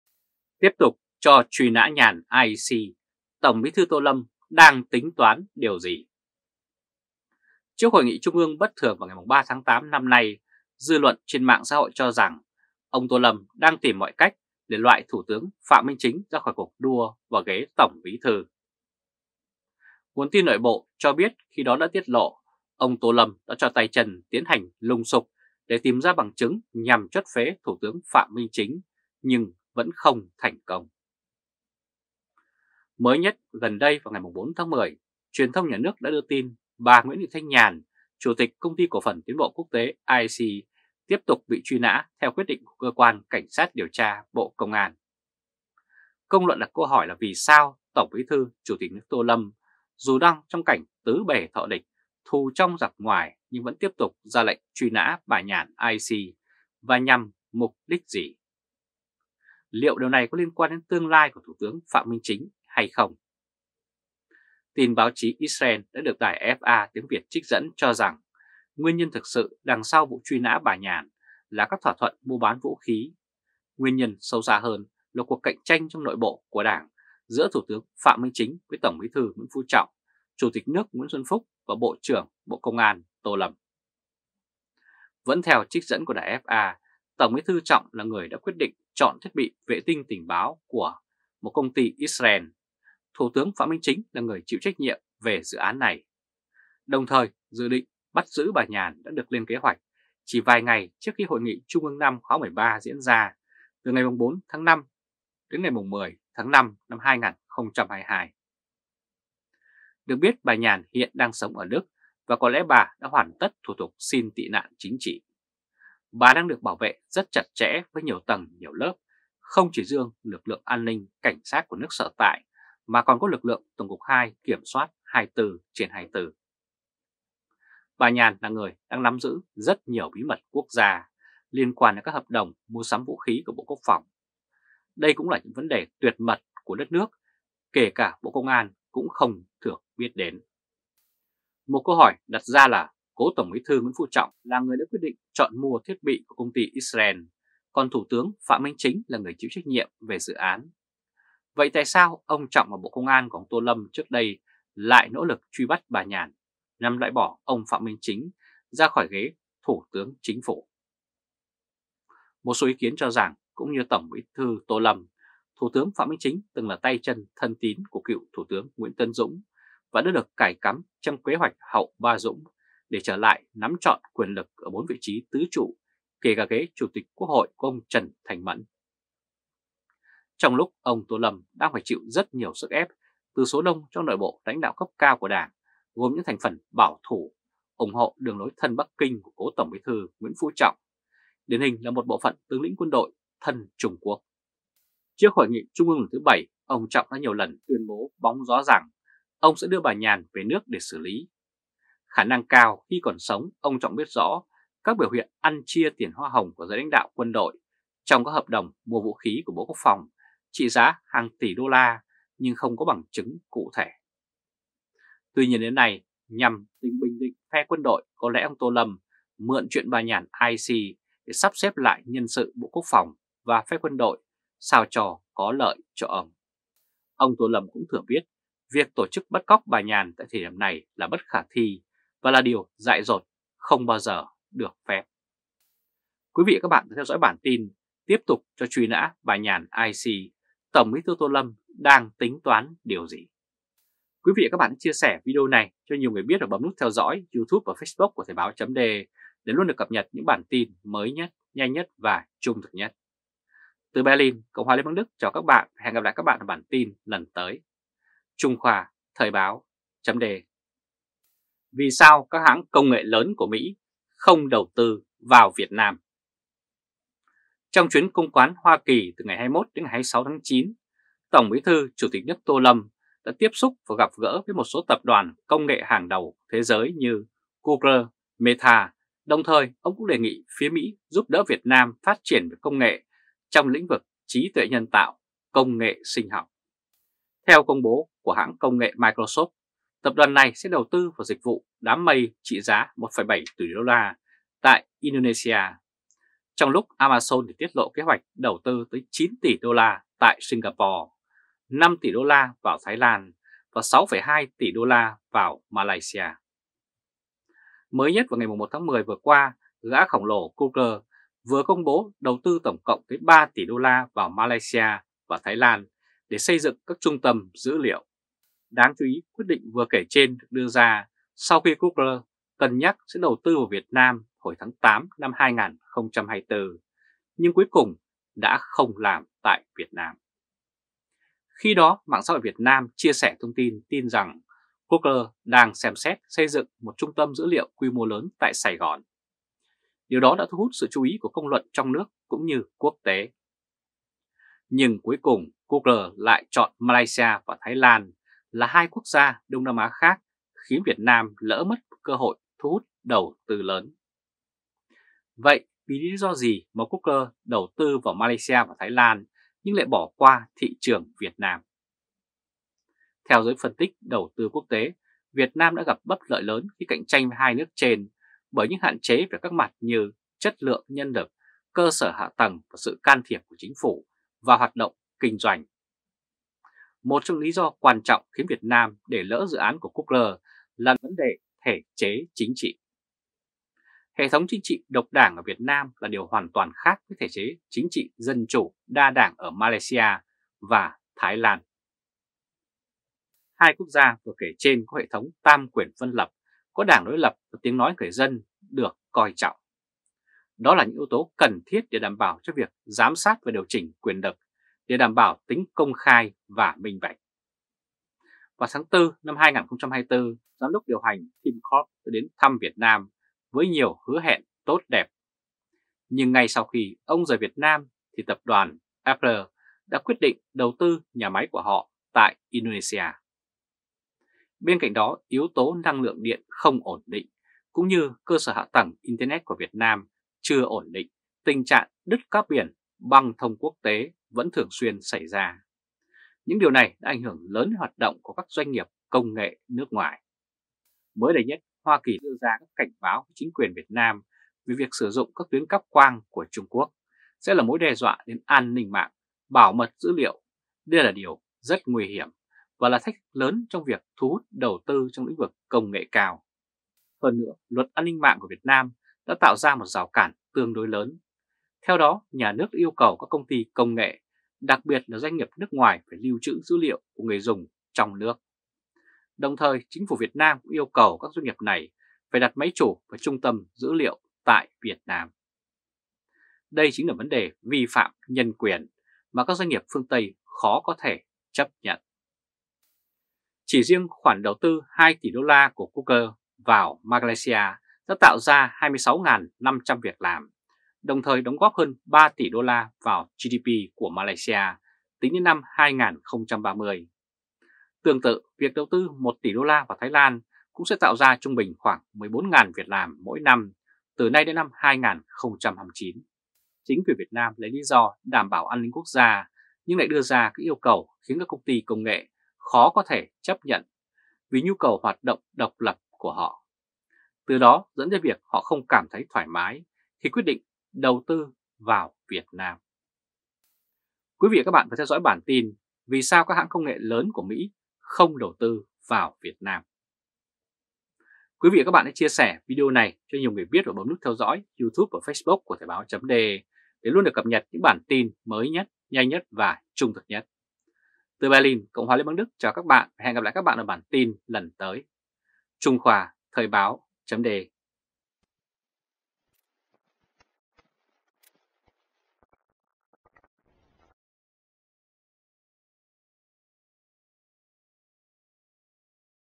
Tiếp tục cho truy nã Nhàn AIC, Tổng Bí thư Tô Lâm đang tính toán điều gì?
Nhằm bình định phe quân đội, Tổng Bí thư Tô Lâm mượn chuyện bà Nhàn AIC, để sắp xếp lại nhân sự của Bộ Quốc phòng và phe quân đội. Ông thừa biết, việc tổ chức bắt cóc bà Nhàn tại thời điểm này là bất khả thi, và là điều dại dột, không bao giờ được phép làm.

Xem bài chi tiết tại:
Hoặc: